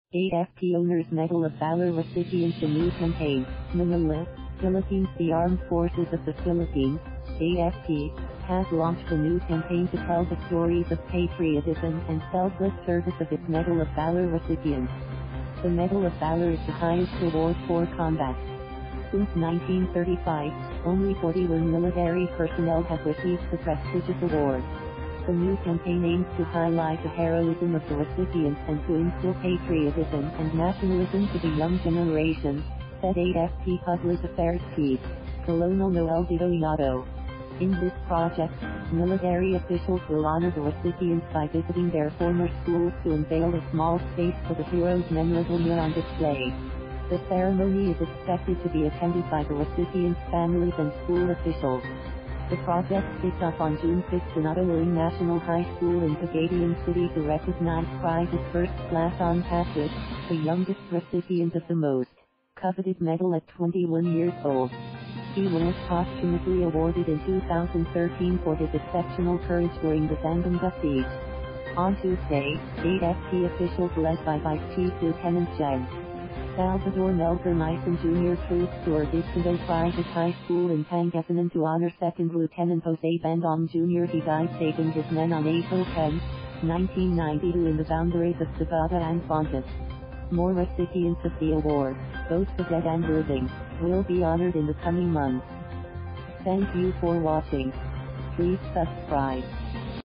AFP honors Medal of Valor recipients in new campaign
MANILA, Philippines — The Armed Forces of the Philippines (AFP) has launched a new campaign to tell the stories of patriotism and selfless service of its Medal of Valor recipients.  The Medal of Valor is the highest award for combat. Since 1935, only 41 military personnel have received the prestigious award. The new campaign aims to highlight the heroism of the recipients and to instill patriotism and nationalism to the young generation, said AFP public affairs chief Col. Noel Detoyato. In this project, military officials will honor the recipients by visiting their former schools to unveil a small space for the heroes’ memorabilia on display. The ceremony is expected to be attended by the recipient’s families and school officials. The project kicked off on June 5 in Otto Lingue National High School in Pagadian City to recognize Private First Class Ian Paquit, the youngest recipient of the most coveted medal at 21 years old. He was posthumously awarded in 2013 for his exceptional courage during the Zamboanga siege. On Tuesday, AFP officials led by Vice Chief Lt. Gen. Salvador Melchor Mison Jr. trooped to Urbiztondo Private High School in Pangasinan to honor Second Lieutenant Jose Bandong Jr.  He died saving his men on April 10, 1992 in the boundaries of Sagada and Bontoc. More recipients of the award, both the dead and living, will be honored in the coming months. (...